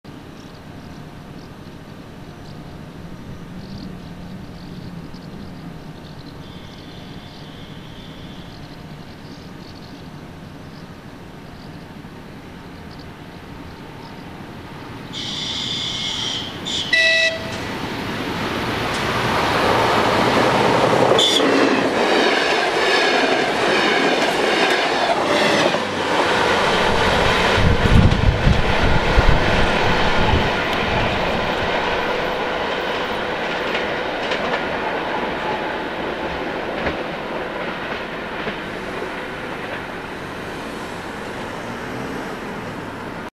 Дизель-поезд ДР1А-230 на ст. Ракке / DR1A-230 DMU passing Rakke station
Дизель-поезд ДР1А-230 на ст. Ракке, Эстония, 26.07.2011
DR1A-230 DMU passing Rakke station, Estonia, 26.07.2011